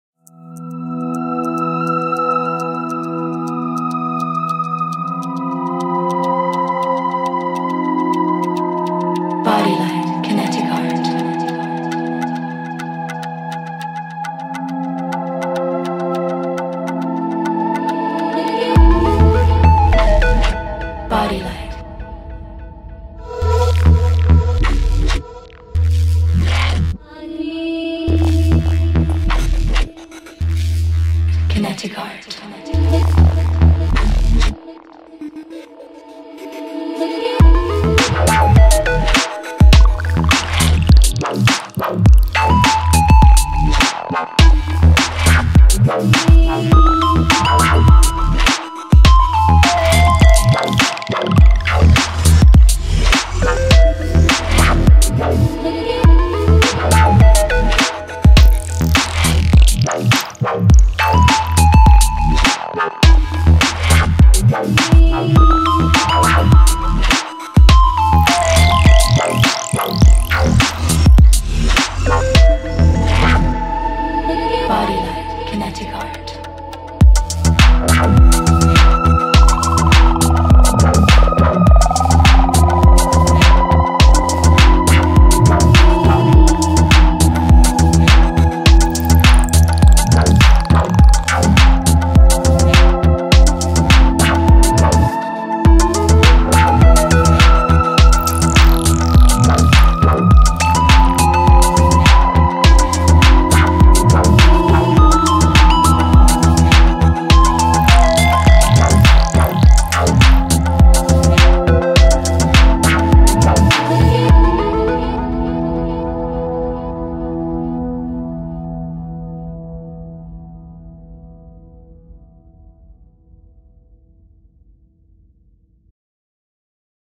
Meet up Merzouga. Body painting and light painting.
Petit retour sur notre cession marocaine où notre MUA, Sita Vilaça a été invitée pour participer à une rencontre internationale de Lightpainting. Elle a créée des créatures pour les photographes.